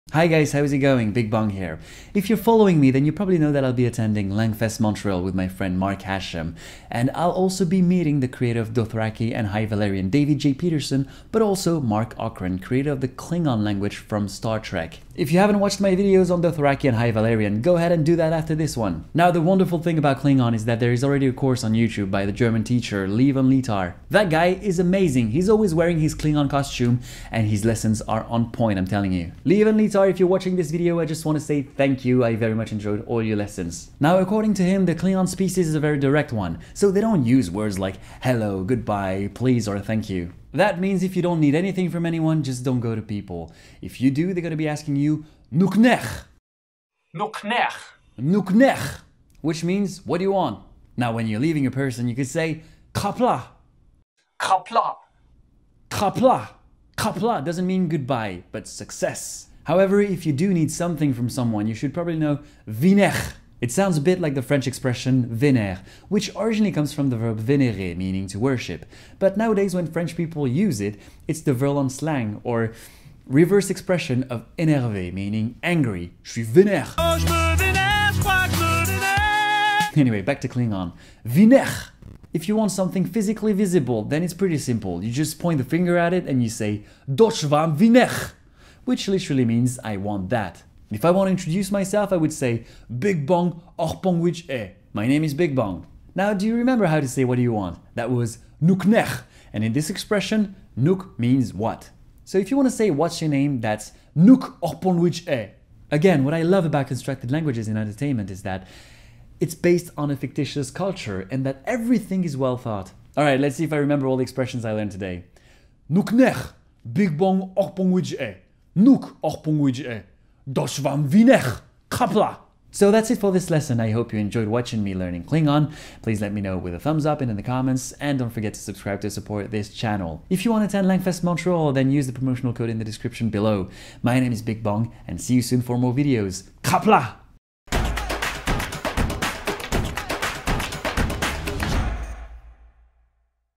Basic Expressions in Klingon - BigBong
Introduce yourself in the constructed language created by Marc Okrand for the world-famous show Star Trek!